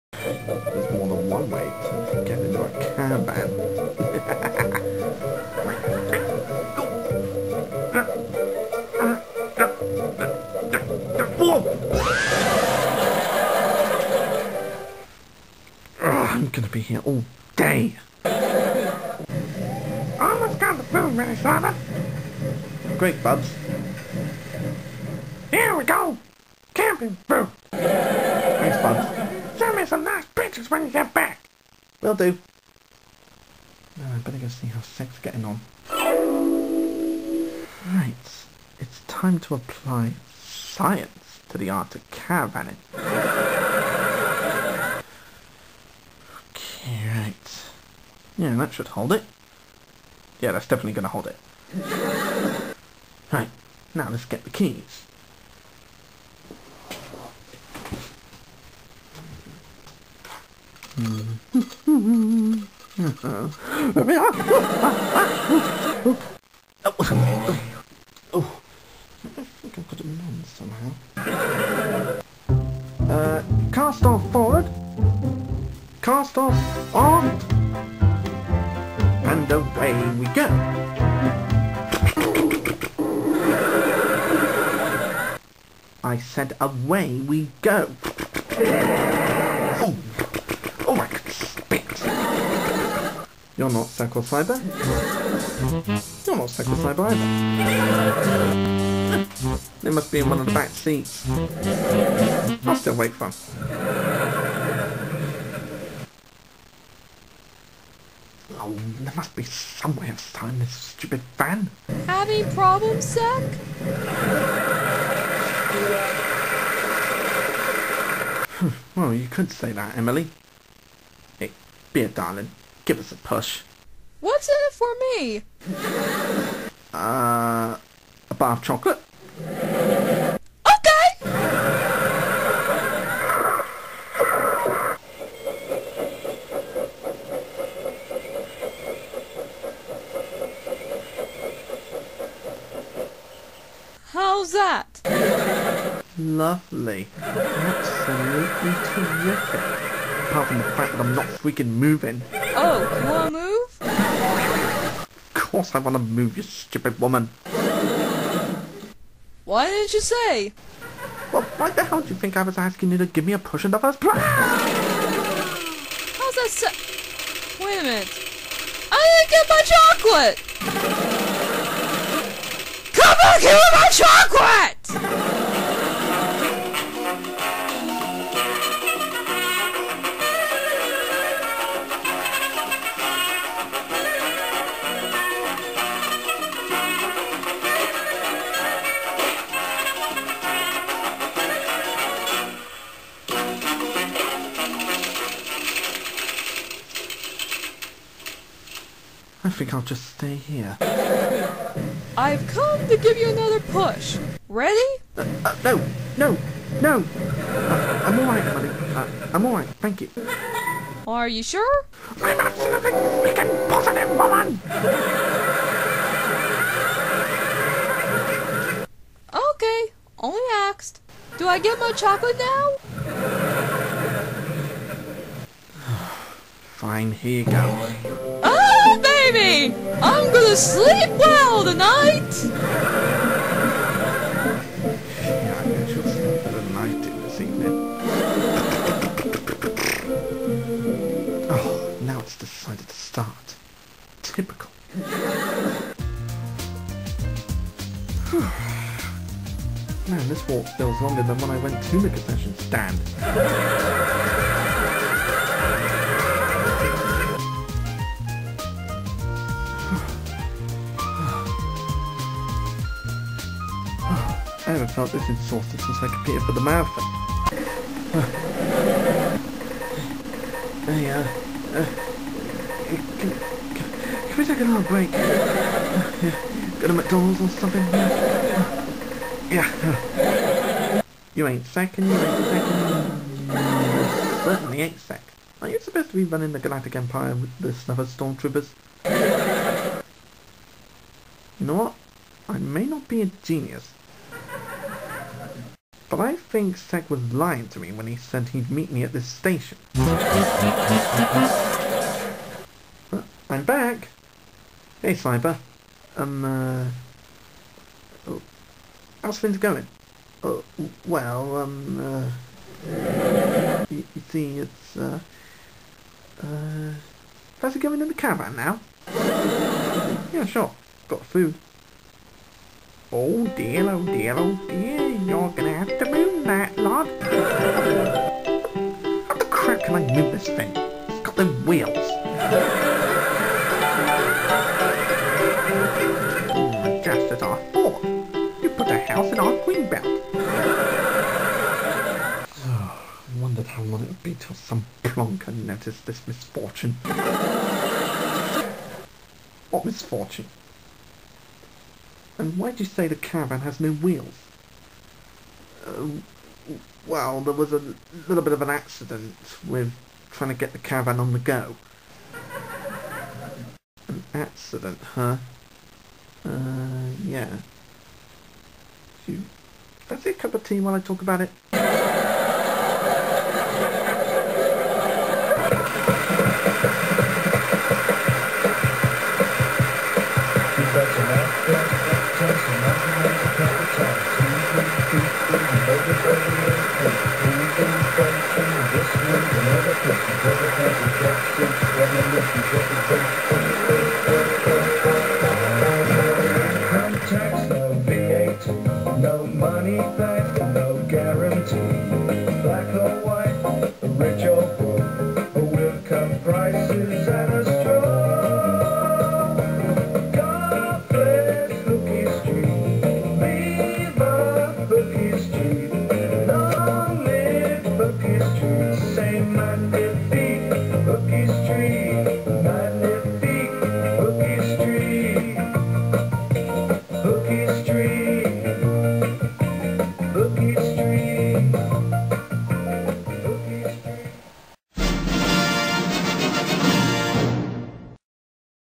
Sec & Co. - Caravan Part 2
Sec still struggles to sort out the caravan. Soon Emily shows up and gives him a hand for a bar of chocolate.

Based on The Magic Roundabout Caravan